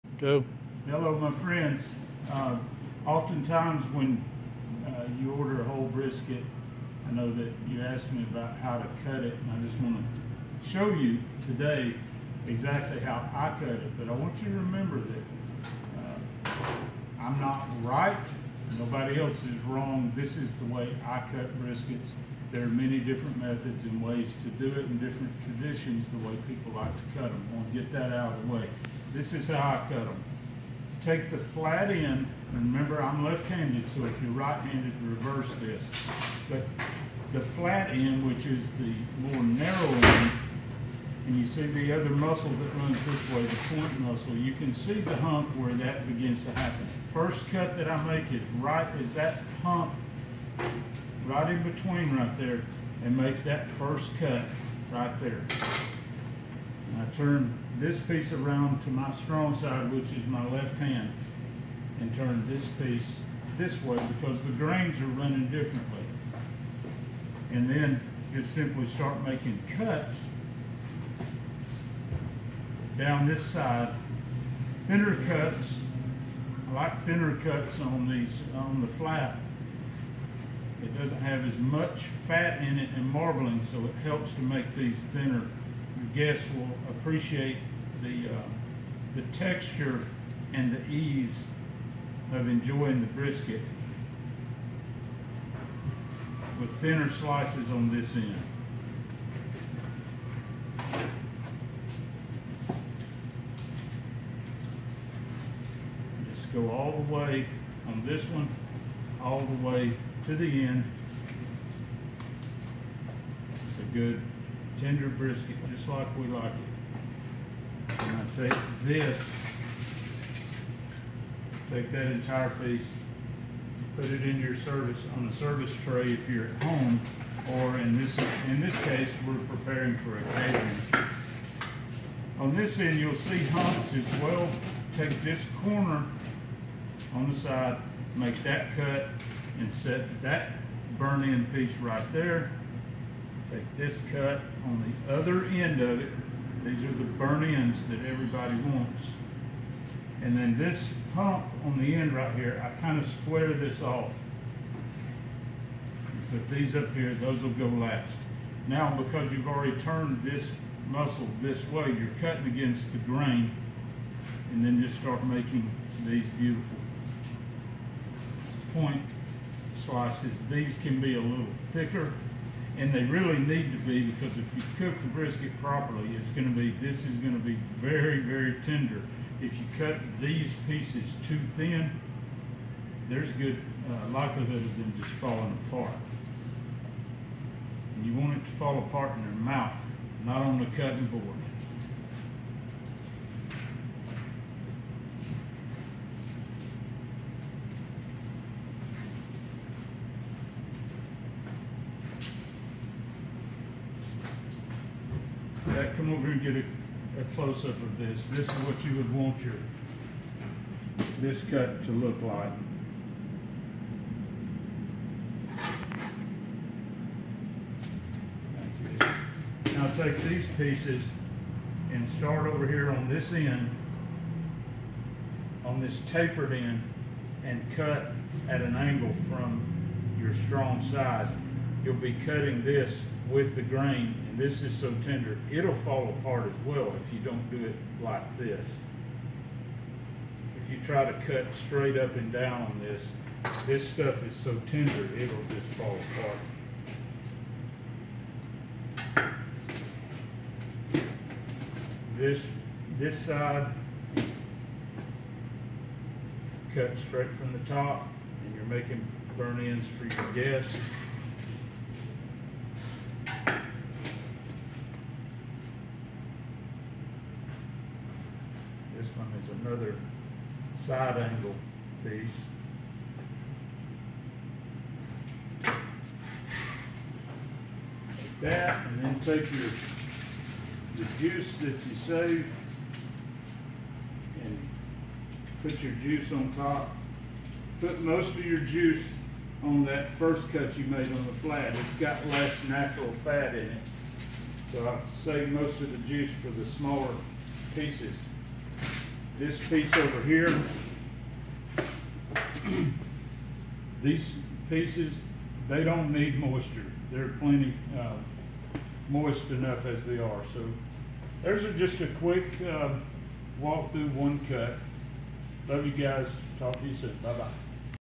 🔥Cutting Whole Brisket🔥
In this video Robert from Wardlaw's Lucky Eye Q demonstrates how LEQ cuts it's briskets at the restaurant (and at home)